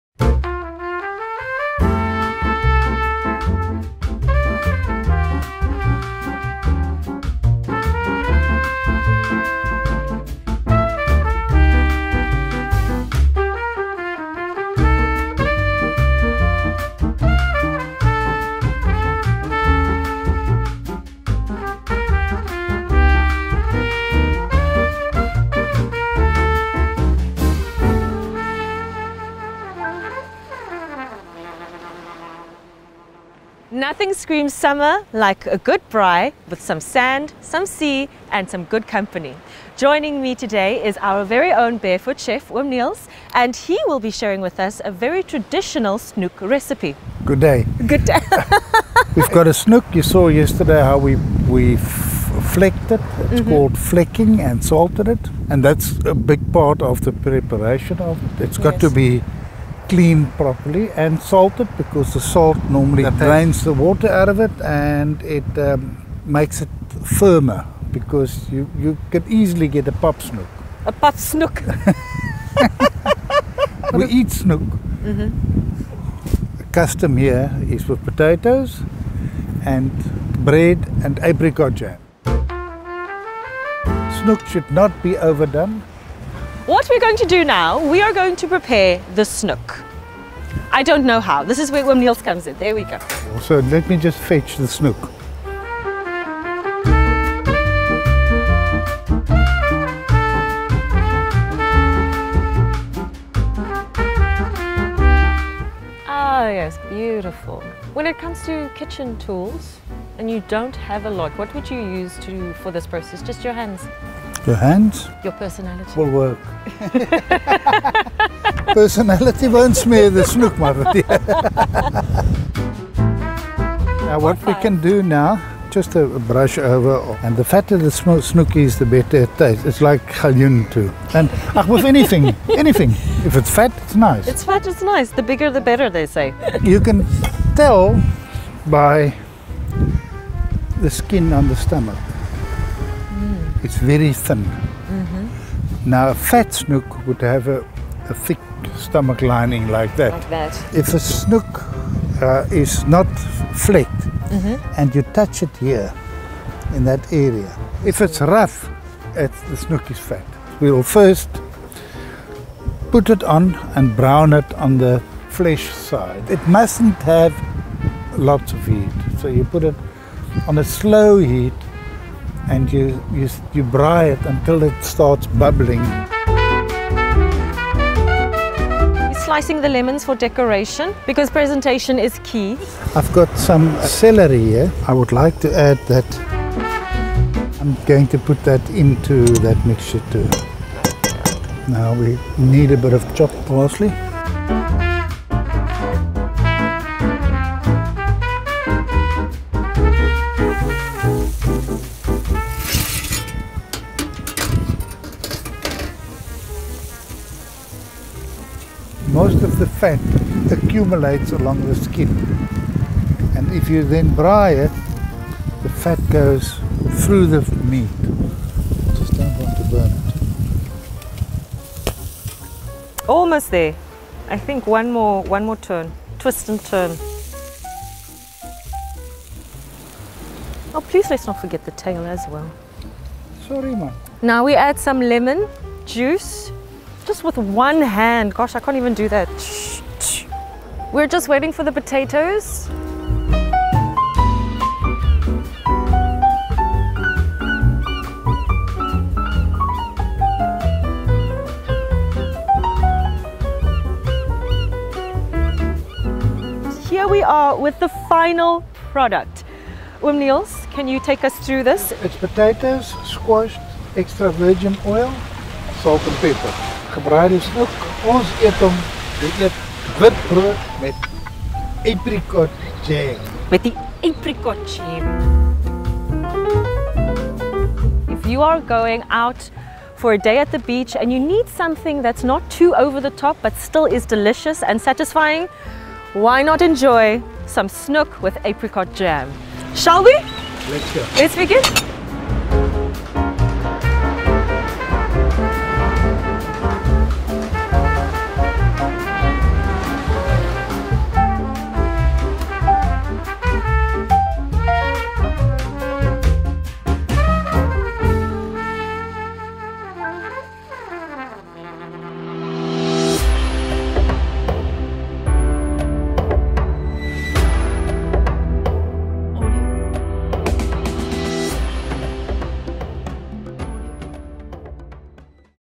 Namibian Recipes - Snoek On The Beach
Join the Gondwana crew as they enjoy a delectable meal on the Namibian coast.

Snoek is a fish enjoyed traditionally on a Namibian fire. The Namibian coastline is the perfect place to enjoy a fresh meal with friends.

 Our chef, oom Neels, adds his Namibian charm when it comes to preparing the snoek.

Join us as we grill our snoek, followed by some lighthearted banter and the sound of the Atlantic on the Namibian coastline.

Website & Newsletter